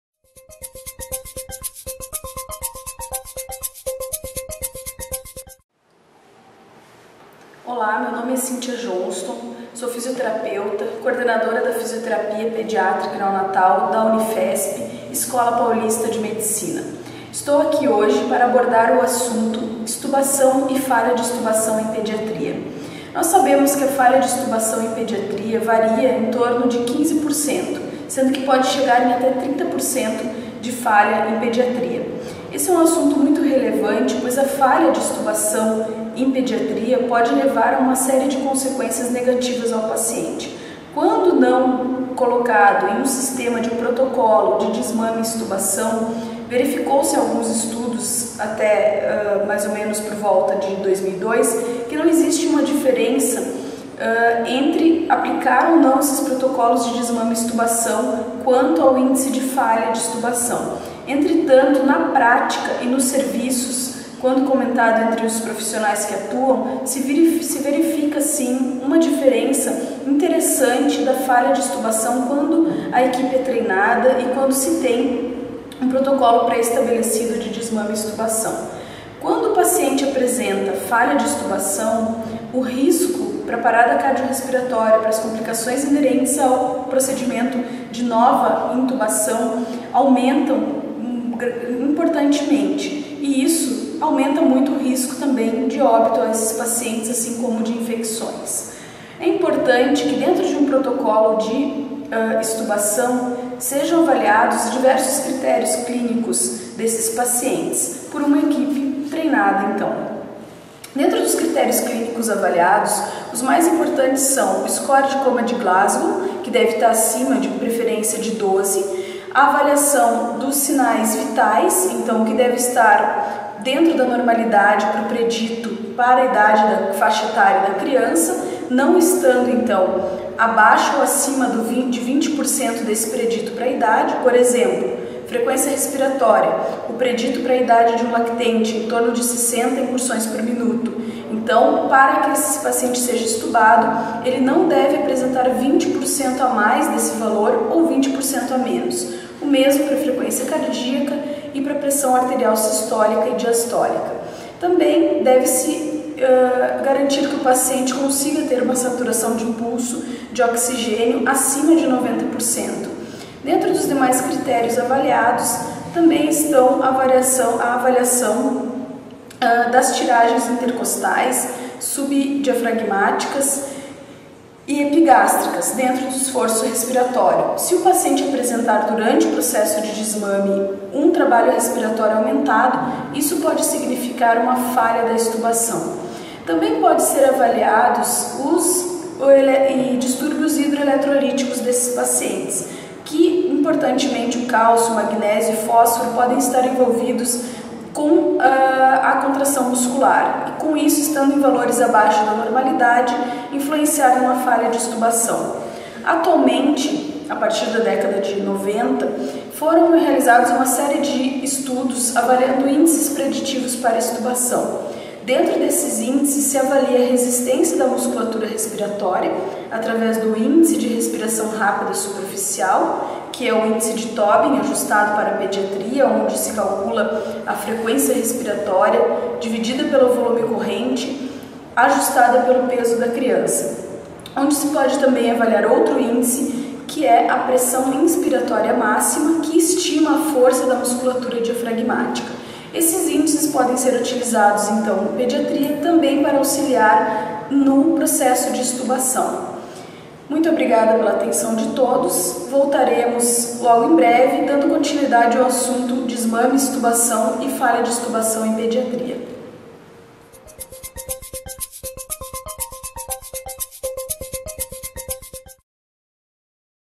VNI - Extubação e Falha da Extubação em Pediatria - Cíntia Johnston
Cíntia Johnston é e autora dos livros: Ventilação Não-Invasiva em Neonatologia e Pediatria; Monitorização e Suporte Hemodinâmico; Fisioterapia e Insuficiência Ventilatória Aguda, todos da Série Terapia Intensiva Pediátrica (Editora Atheneu). Atualmente coordena a Fisioterapia Pediátrica Neonatal da UNIFESP - Escola Paulista de Medicina.

Video trata de aspectos relacionados a extubação e falha da mesma, assim como os critérios clínicos para a extubação.